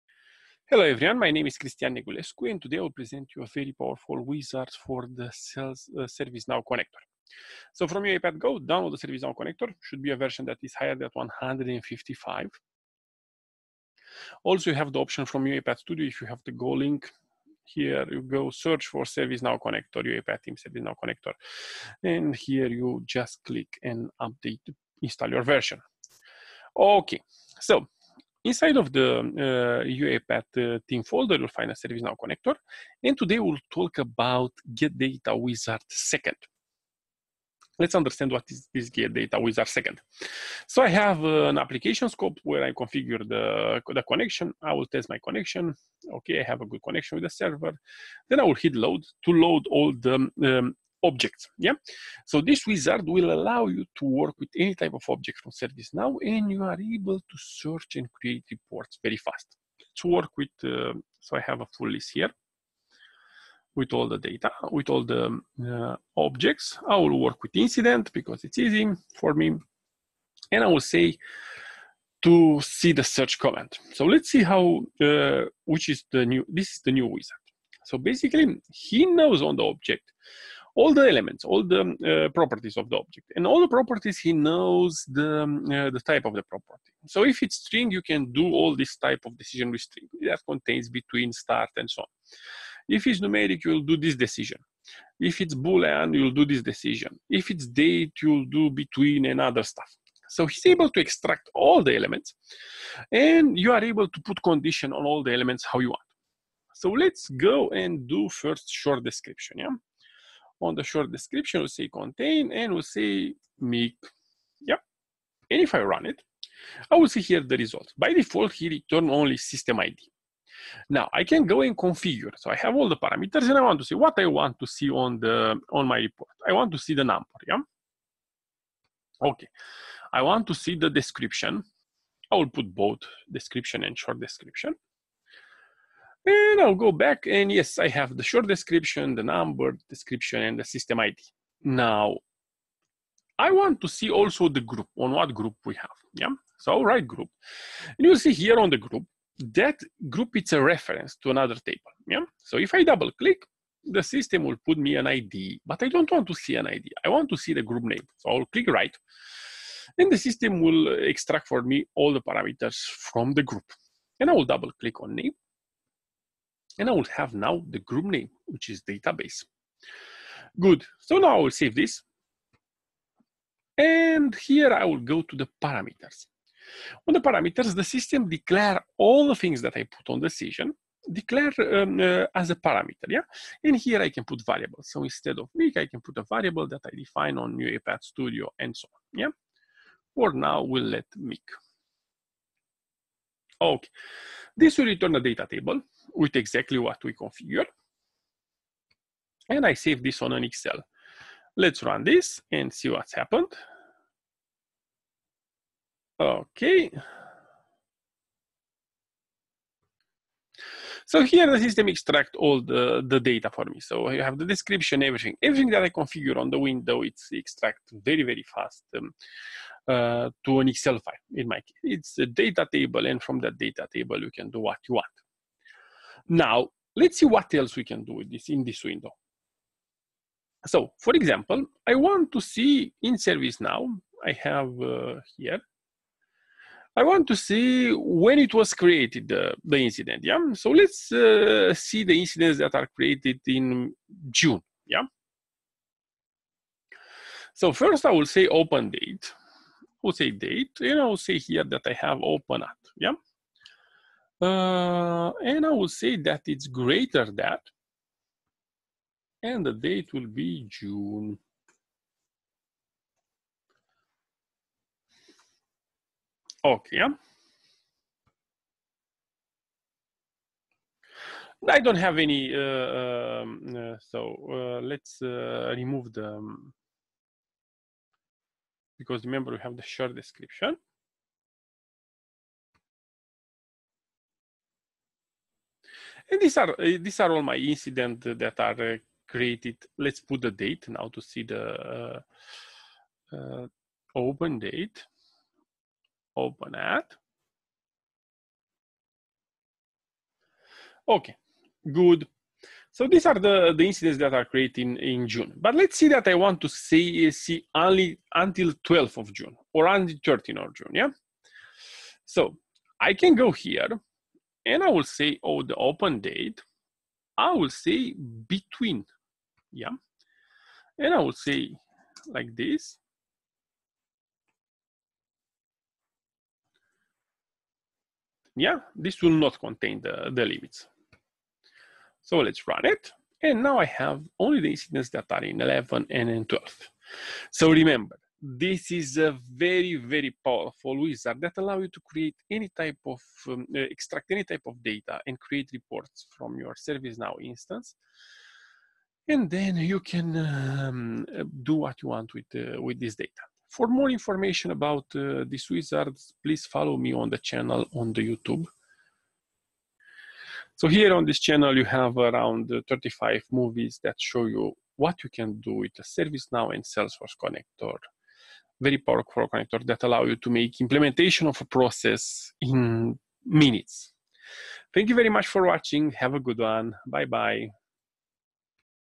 UiPath Get Data from ServiceNow | RPA | Wizard | Inteligent filters | Extract to datatable
A brand new powerful wizard that allow getting any data from ServiceNow under 5 minutes (Implementation time)

RPA implementation on ServiceNow | UiPath | Snow | REST API | RPA | General Presentation | Activity

0:00 Intro
0:40 Get data wizard 2 activity
1:30 Decision type
2:00 Configure output
2:55 Edit Parameters
3:20 Run the Robot
4:10 Get the incident between a period